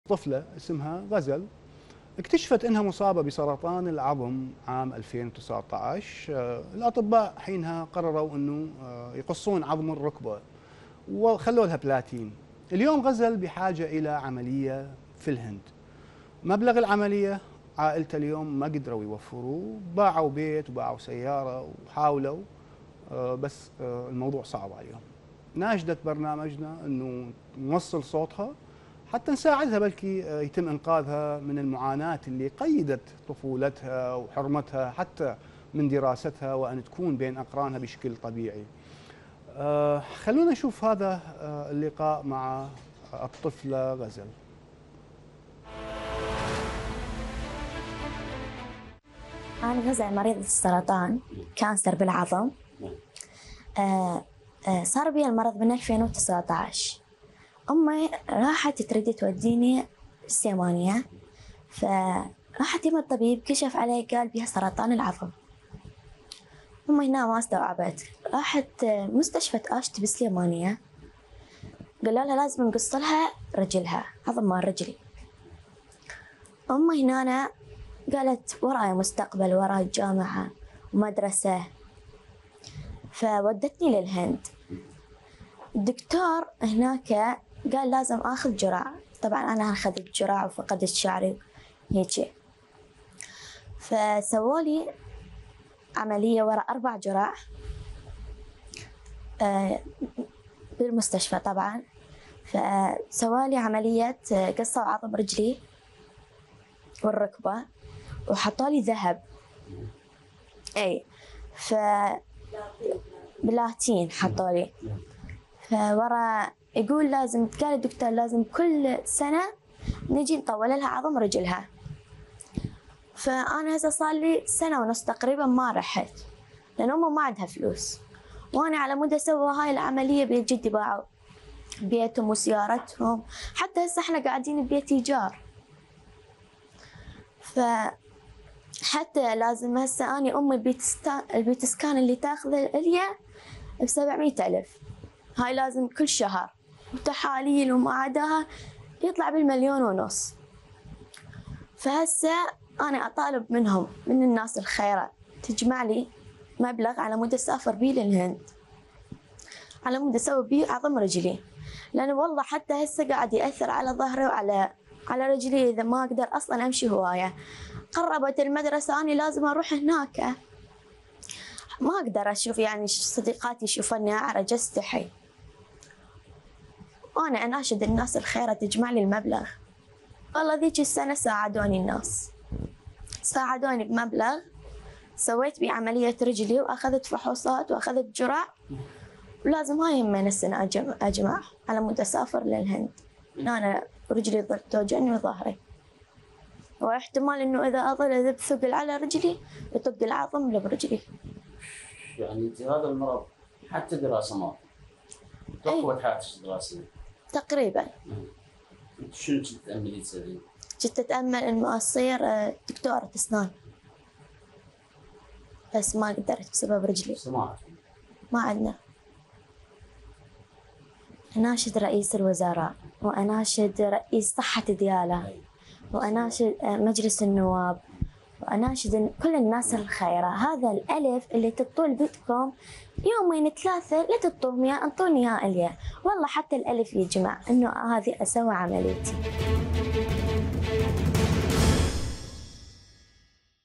عاجل .. طفلة عراقية مصابة بهذا المرض الخطير ولا تملك ثمن العلاج
موقع قناة التغيير الفضائية الالكتروني